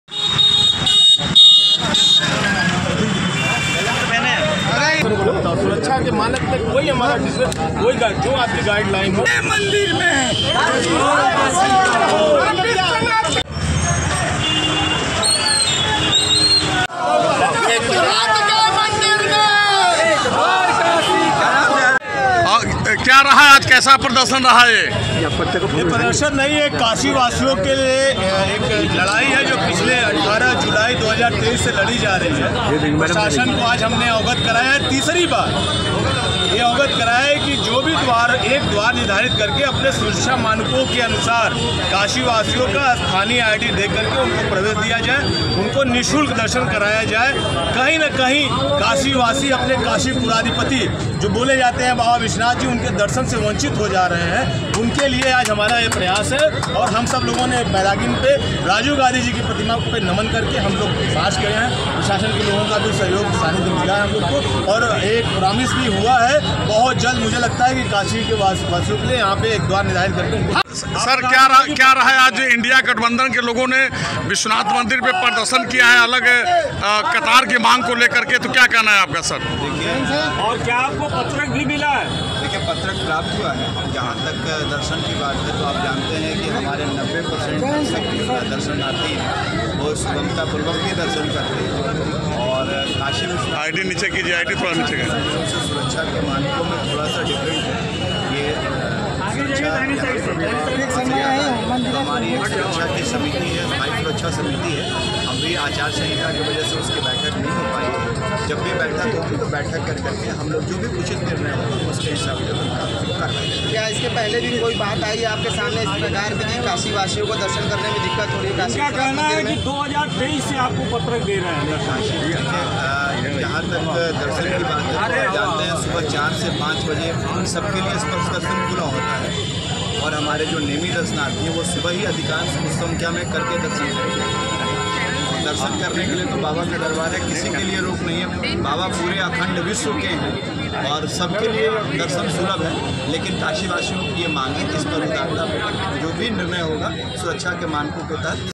INDIA गठबंधन का प्रदर्शन, काशीवासियों के लिए की ये मांग | Varanasi | PM Modi | Modi 3.0
विश्वनाथ धाम में काशीवासियों के लिए दर्शन के लिए एक द्वार निर्धारित करने की मांग को लेकर इंडी गठबंधन ने प्रदर्शन किया। वाराणसी में मैदागिन से लेकर के विश्वनाथ मंदिर गेट नंम्बर 4 तक तक पदयात्रा निकाली गई।